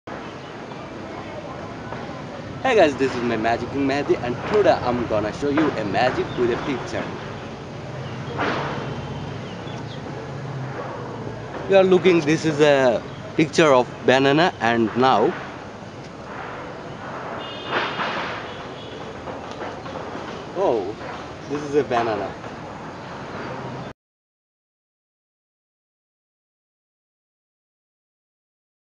MAGIC KING mehedi Funny vines (glamor, infatuation, magick)
Hello dear,
In this video, I will show you FUNNY MAGIC VINES (glamor, infatuation, magick). Watch the magicians funny wizardry, witchery, and funny juggling magic. I am the funny sorcerer, MAGIC KING MEHEDI the king of magic.